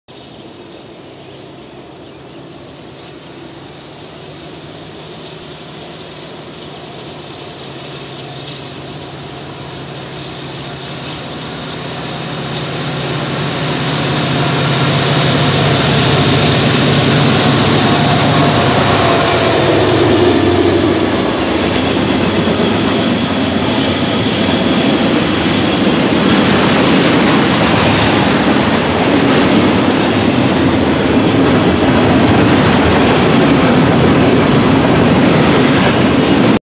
NS Illinois Terminal heritage unit leads train #294 at Otis, IN
Heading east. 7/16/19.